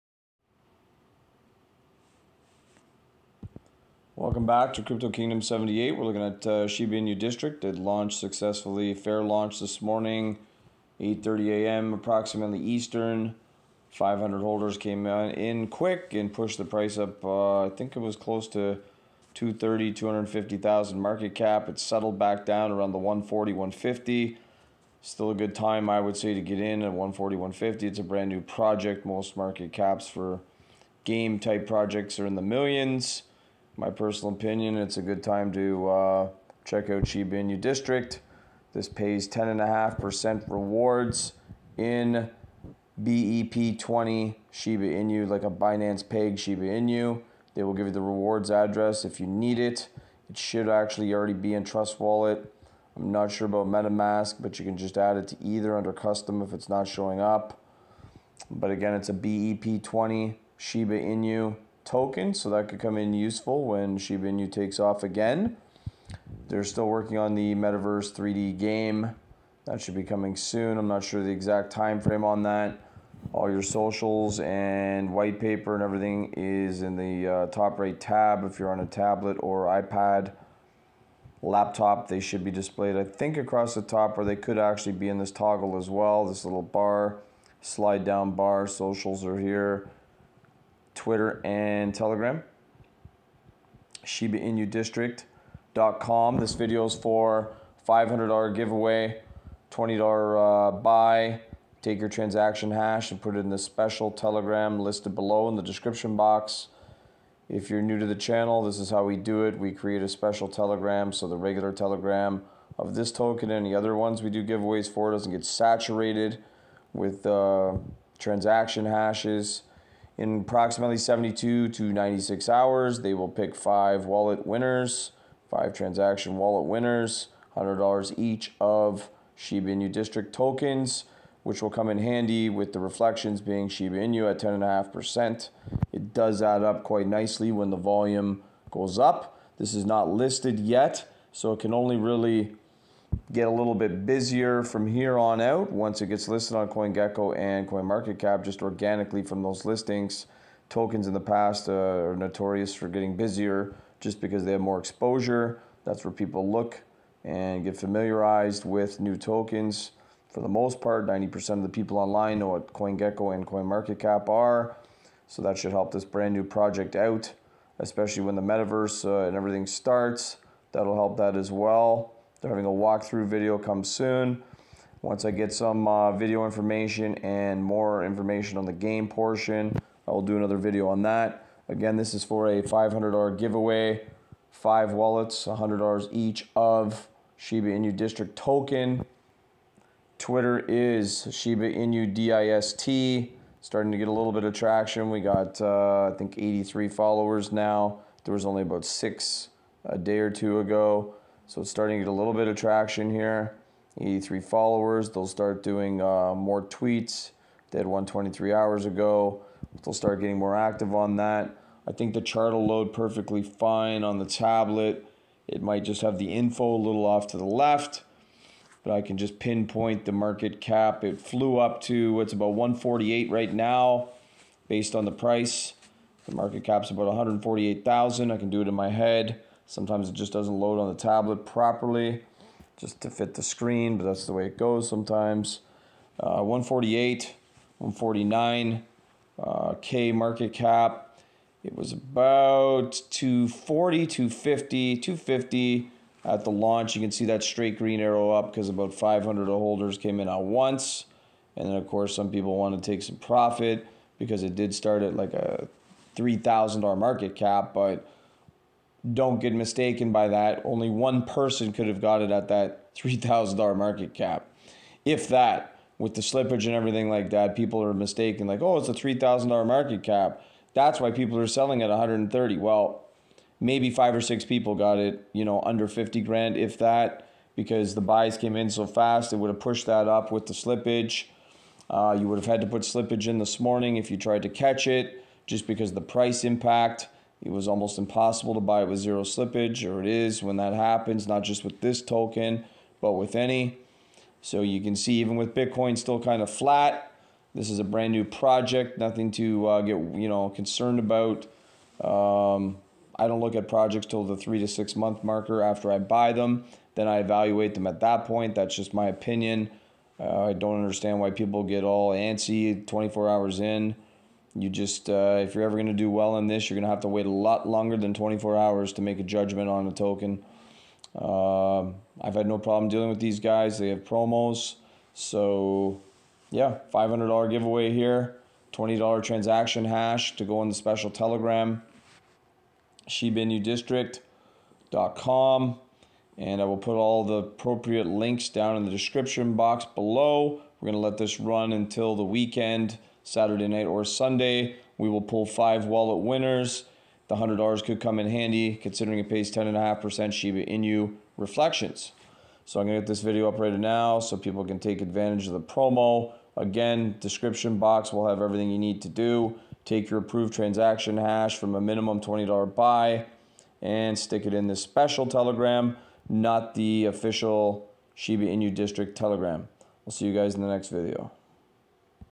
SHIBA INU DISTRICT UPDATE BEP20 Launched 3D Metaverse + 10.5% Shiba Reflection + 5 x $100 Giveaway
GIVEAWAY DETAILS: Please take your trans hash (min buy $20usd) and place it in the Special Telegram

*All My Videos here are for Entertainment & Help Purposes Only! Please everyone do your own research in this space*